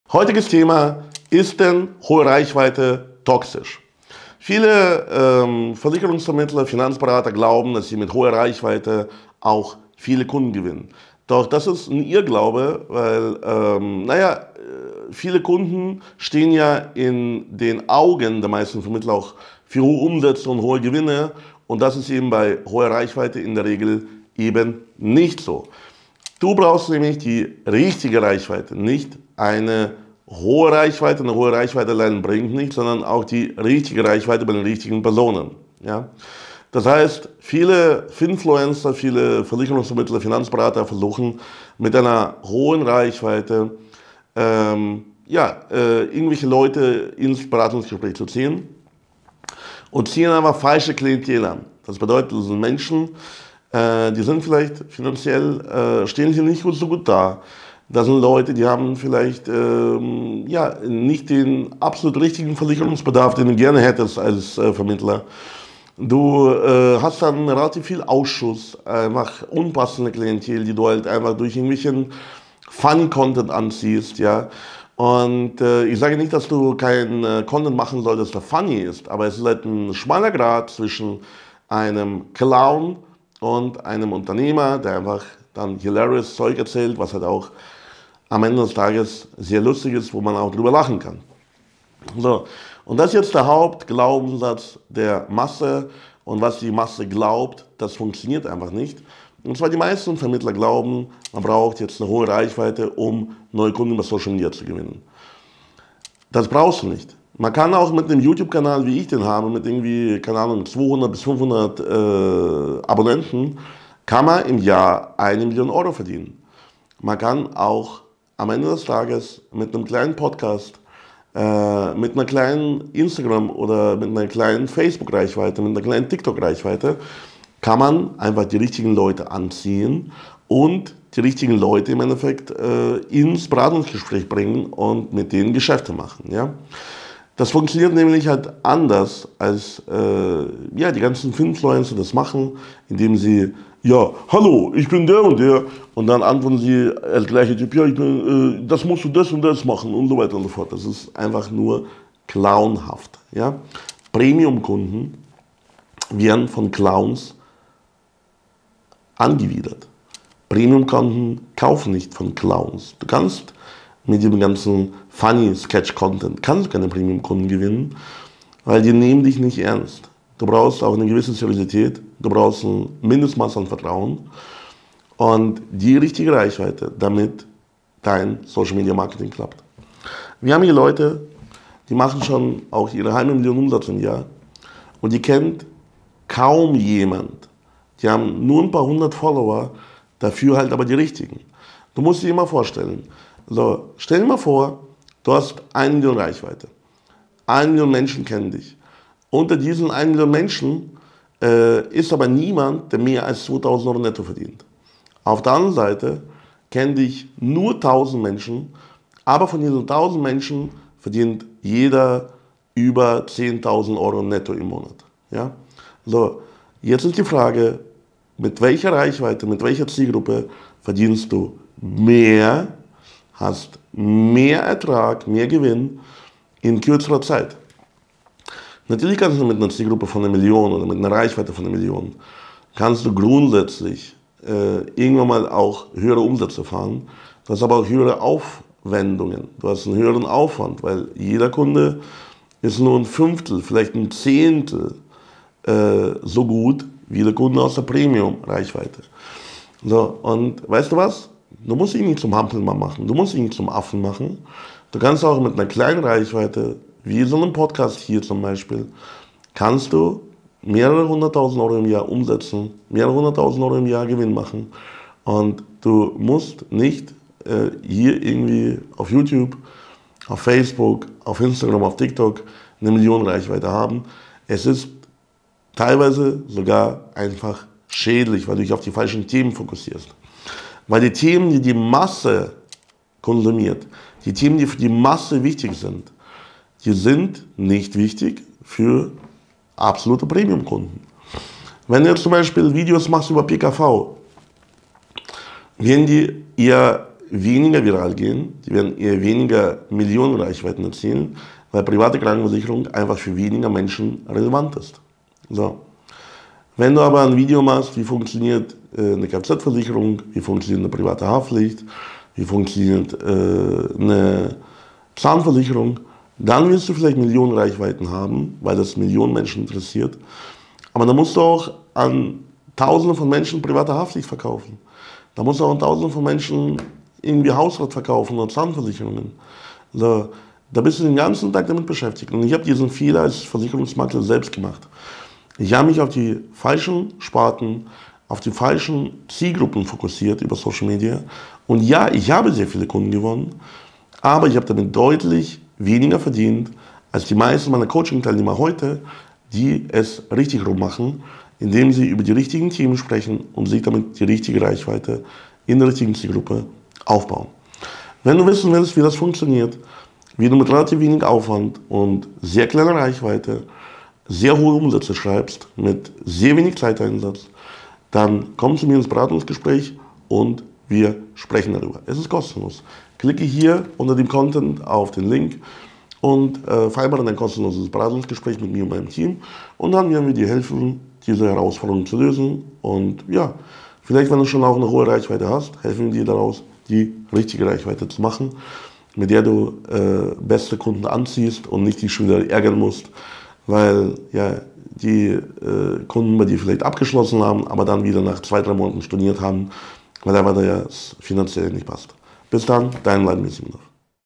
Tanzende Finanzberater verlieren Kunden
In diesem Video spricht Wladimir Simonov über das Thema "Hohe Reichweite" und warum dies nicht der richtige Lösung für Finanzberater ist.